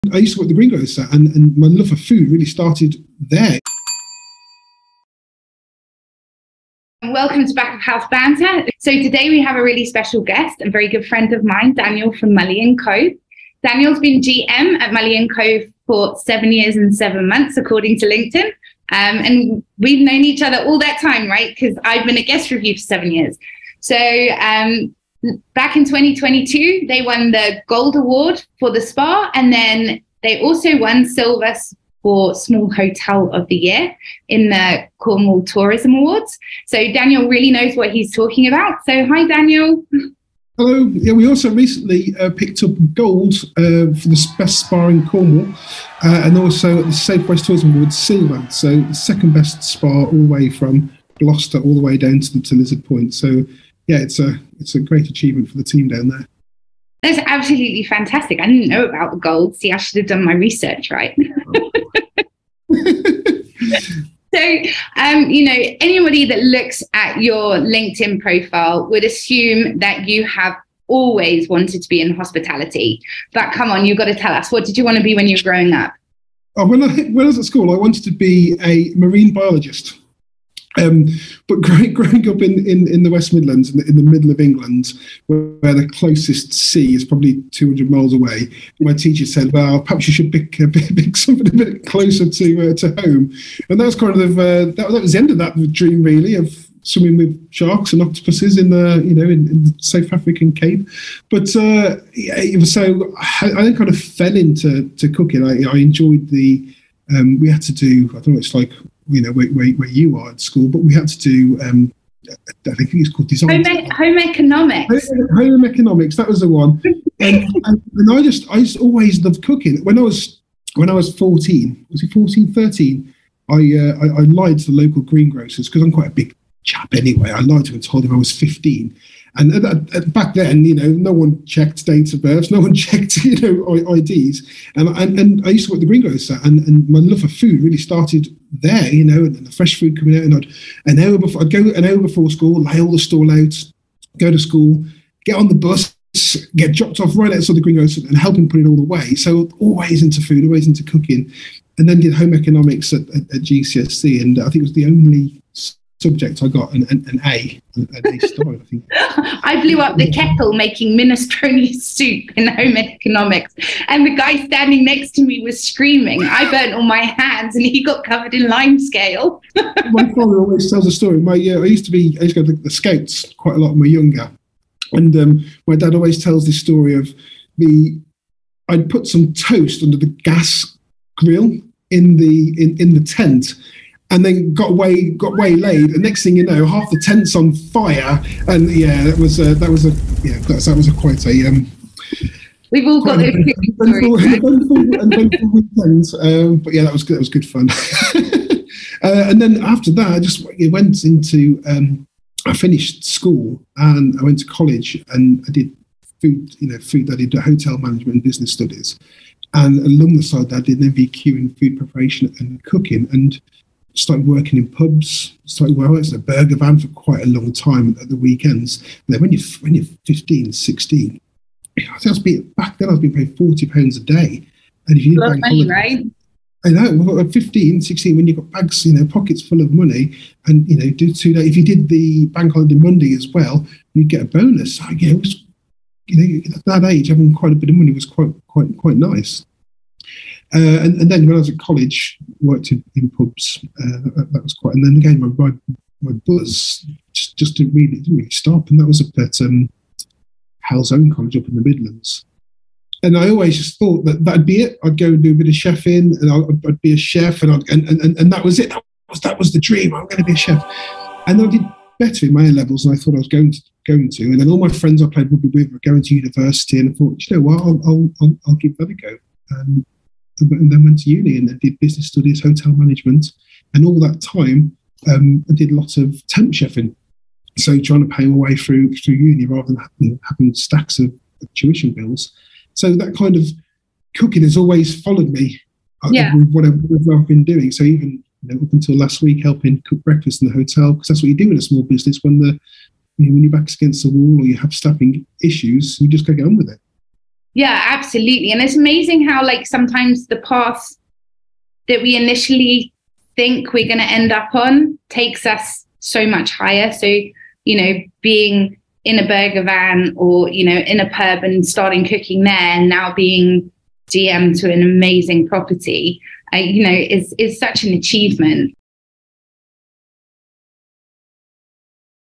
Building a Dream Team with Daniel (Part 1: Meet the grocer’s lad who became a GM)
Daniel discovered his love of food when he began working for the local greengrocer in his early teens, and cheffing seemed to be something he just couldn't shake off. In this episode of Back of House Banter, Daniel tells Amy how he still occasionally helps out in the restaurant of the award-winning Mullion Cove Hotel he now runs.